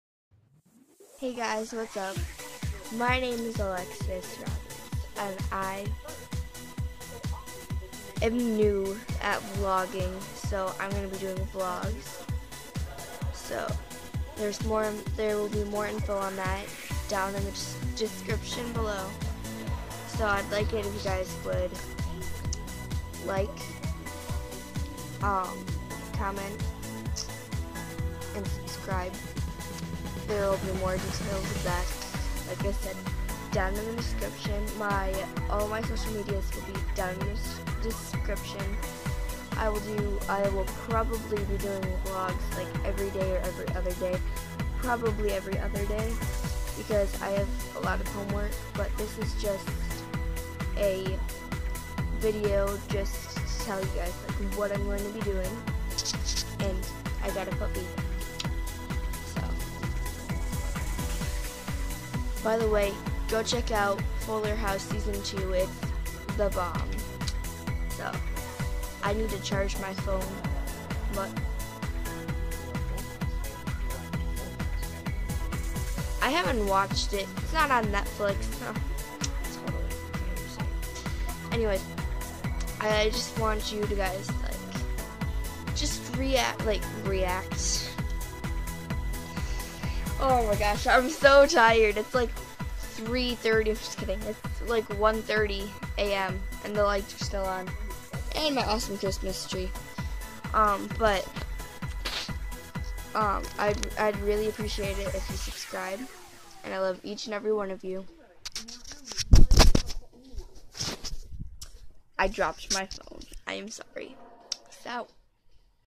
I'm doing vlogs for the first time lol
So I will be doing vlogs everyday and cool stuff w/ it. My social medias are insta = alexis.